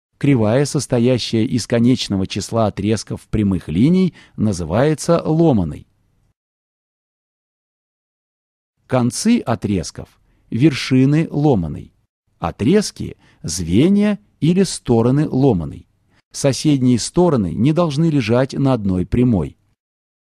ЛОМАНАЯ. Геометрия 7 класс
Кривая, состоящая из конечного числа отрезков прямых линий, называется ломаной.

Концы отрезков — вершины ломаной.

Отрезки — звенья, или стороны, ломаной. Соседние стороны не должны лежать на одной прямой.